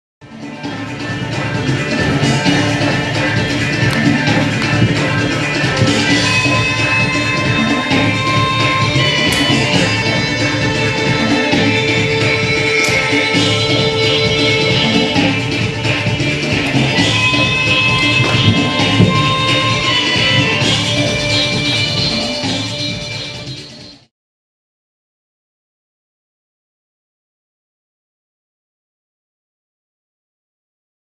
++一千零一夜舞坊~~家帆老師示範非洲舞
一千零一夜舞坊的家帆老師示範非洲舞!!
台北市忠孝東路一段176號五樓之二